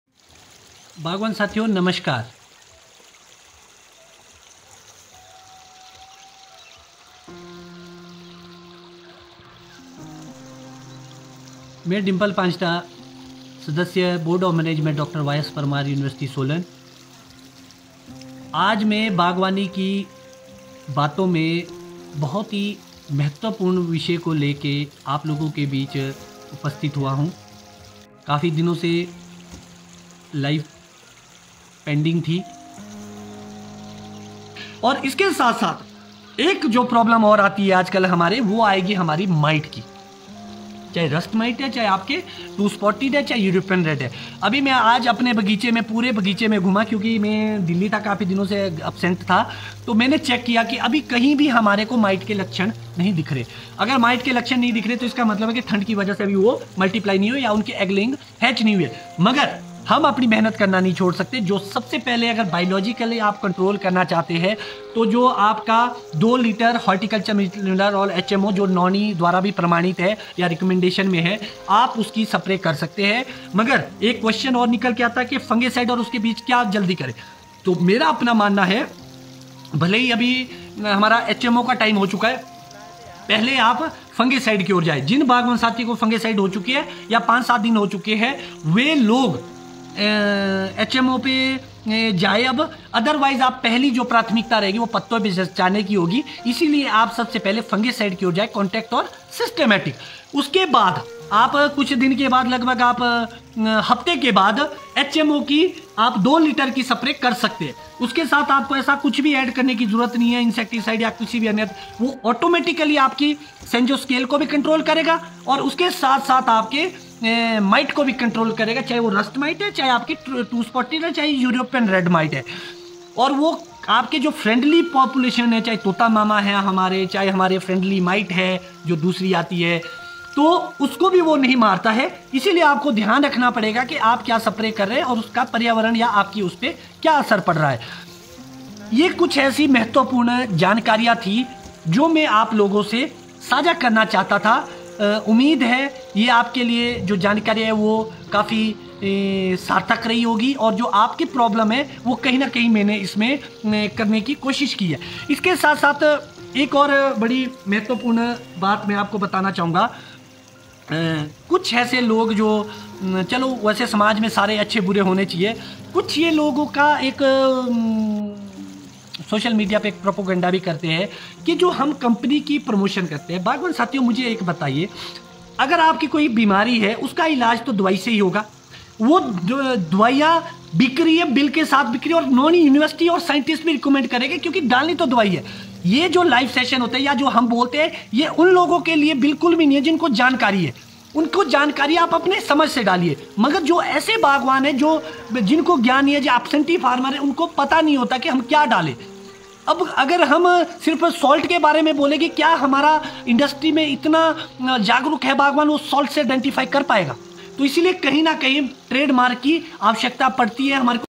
Apple Mite Management | rust mite, 2 spotted mite, European red mite | Himalayan Farming | Live
rust mite
2 spotted mite
European red miteThe apple rust mite has been studied both as a pest and a beneficial arthropod. Although it can damage fruit and foliage of apple, its major role in Pacific Northwest orchards is as an alternate food source for predatory mites.आज के इस विडियो में हम spray schedule के बारे में कुछ महत्वपूर्ण जानकारियां साझा कर रहे हैं।